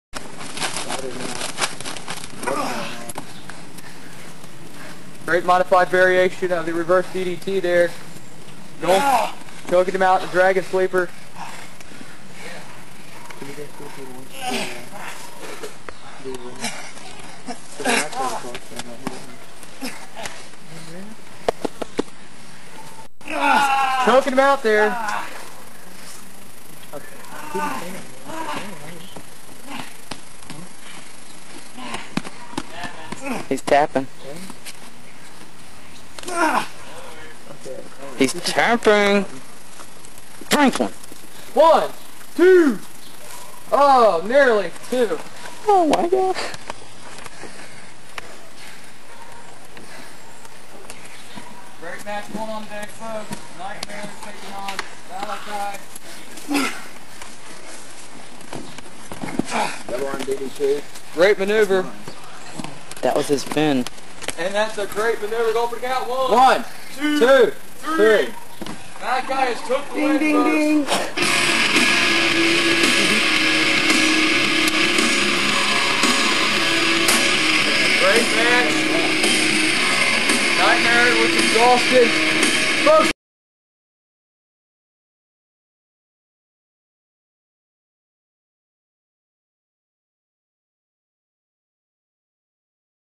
Commentary Botch - Malachi vs. Nightmare
The CWO held their event at the HWF arena and during the last few minutes of this match Budric, the CEO of CWO made a botch not paying attention due his distraction from management behind the scenes.